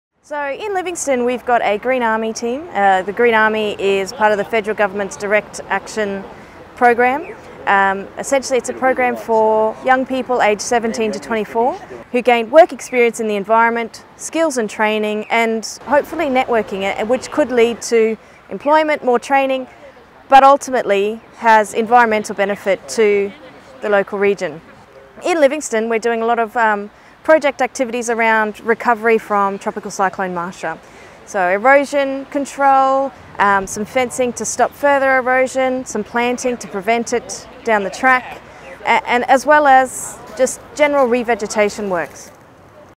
Livingstone Shire joins forces with Green Army
Green thumbs from across Central Queensland have been rolling up their sleeves to help protect some of the region’s most delicate ecosystems.

The team of nine young people aged 17 – 24 years have been working alongside Council’s Natural Resource Team since July and will continue in a number of areas across the Shire for the next four months.

Some of the jobs carried out so far include weed management, replanting the dunes with local species including native spinifex and the construction of a bushland walking track.

Over the course of the projects, the crews will construct more than 2 kilometres of fencing and plant more than 2,000 trees.

The Green Army is an Australian Government initiative that supports local environmental and heritage conservation projects across Australia.

It’s an initiative for young Australians aged 17–24 years interested in protecting their local environment. It provides young people with skills, training and experience to help them enter the workforce or improve their career opportunities.

As a service provider, Conservation Volunteers Australia is currently delivering over 200 Green Army projects around Australia.